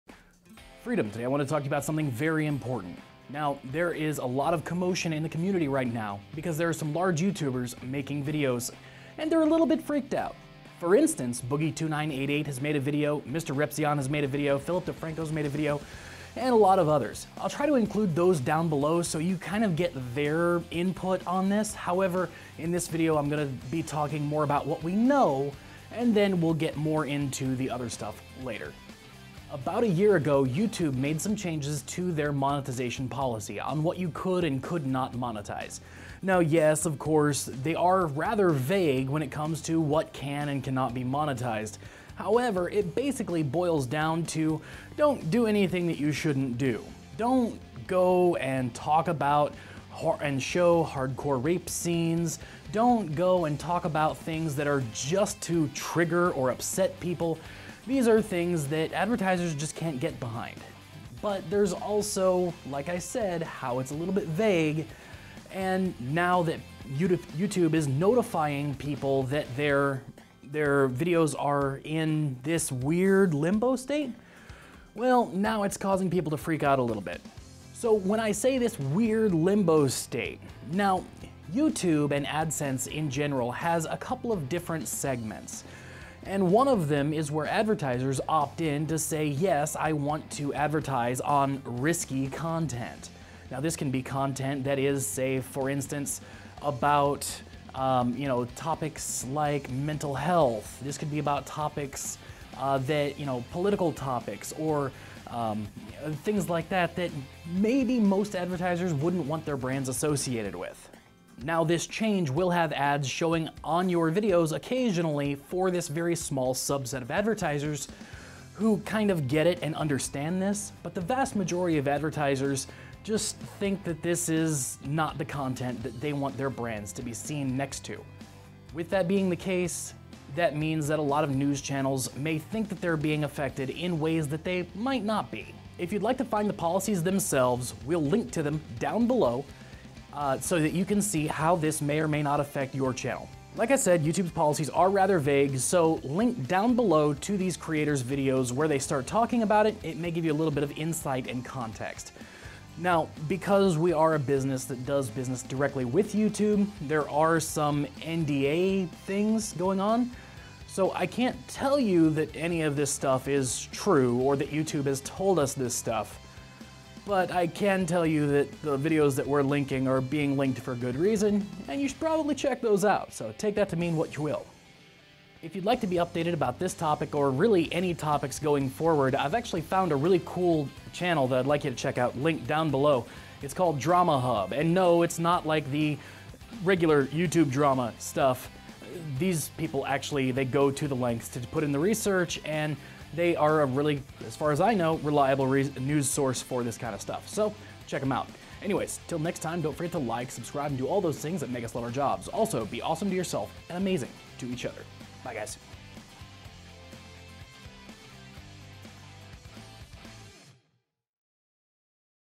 ★ Should we all just quit YouTube and get "real jobs" or is this all just hype?
As usual with these things some large and rightfully scared large YouTubers may have blown this out of proportion. These policies have been in place for a year but are just now coming to the surface with new notifications.

What can you monetize?

Oh NeverGiveUp it's not as bad as it seems!!

▼ Heartbeat

▼ Freedom!

▼ Music